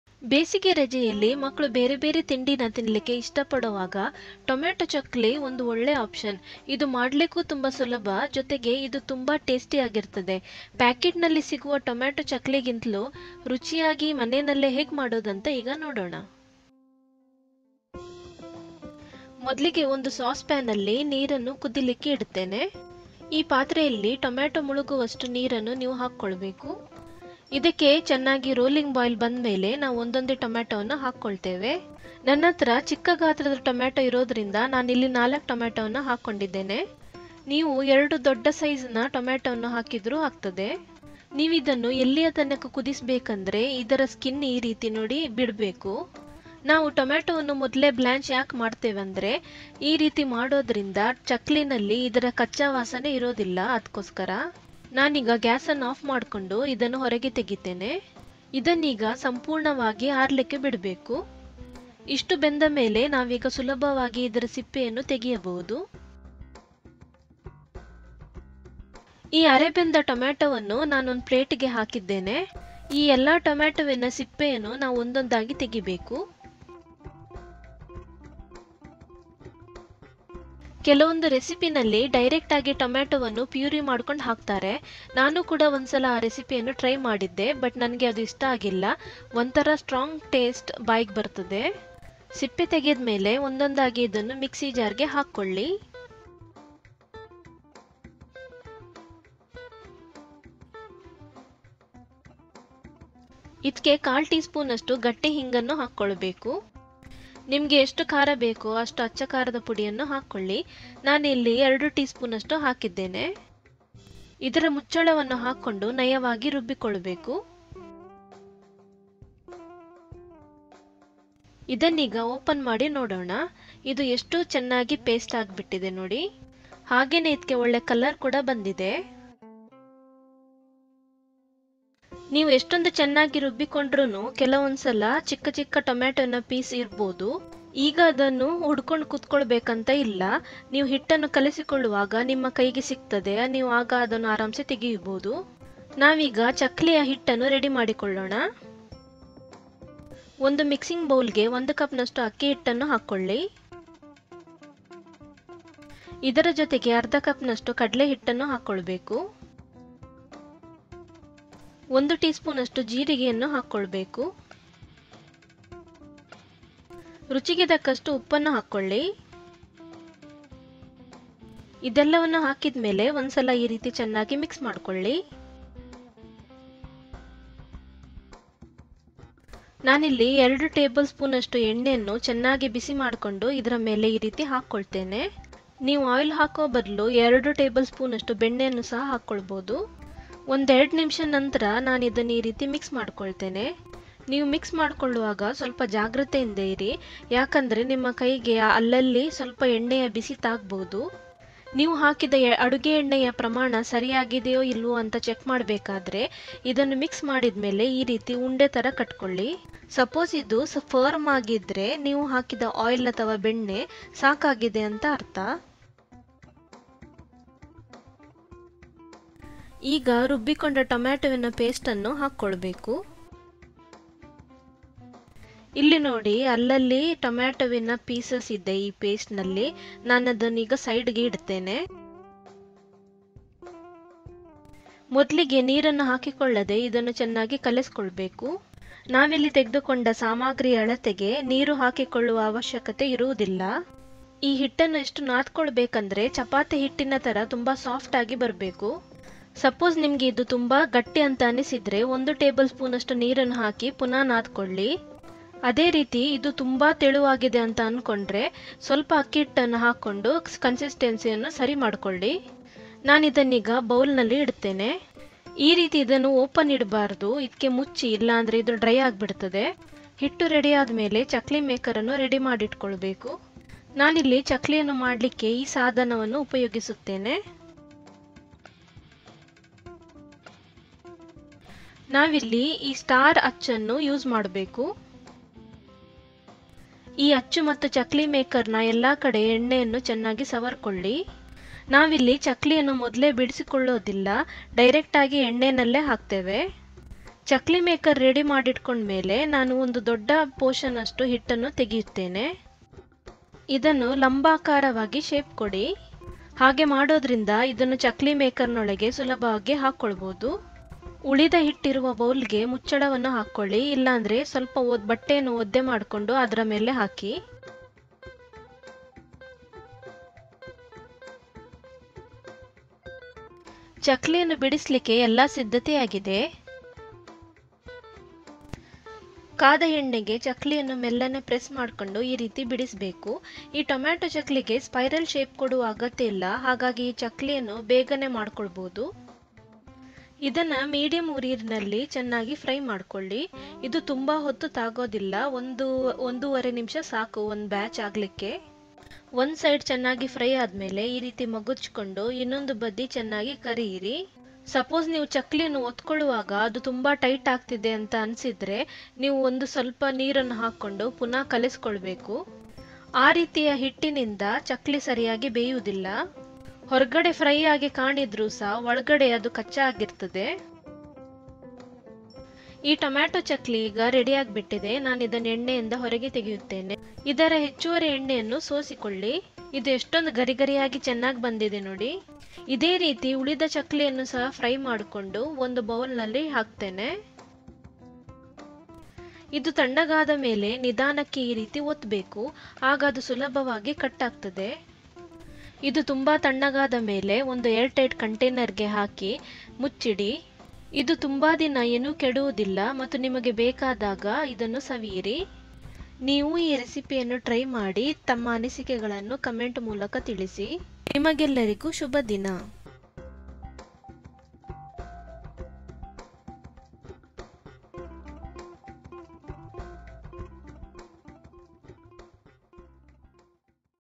Tomato Chakli Recipe - Tomato Murukku - ಟೊಮೇಟೊ ಮುರುಕ್ಕು - Simple Snacks Recipe - HOME CHEFS COOKING
Chakli which is also called as murukku is a most popular snacks from South India. These days we see varieties of recipe of murukku’s in different flavors. In this video you will learn how to make tomato chakli like store bought. Do watch this recipe and let us know your feedback in comments.

Measurement : 1 cup = 250 ml